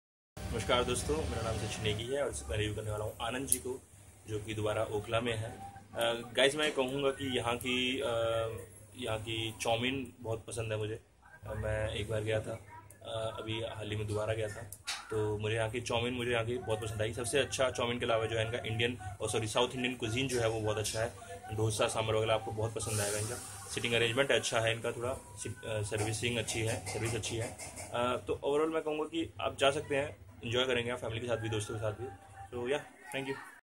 Anand Ji Restaurant, Delhi | Sweets & Savouries
Anand Ji Restaurant is located at 35, Community Centre, New Friends Colony, New Delhi. It is a nice place to have economical meal. They serve North Indian, Chinese and Street Food. Chinese food is the major plus point.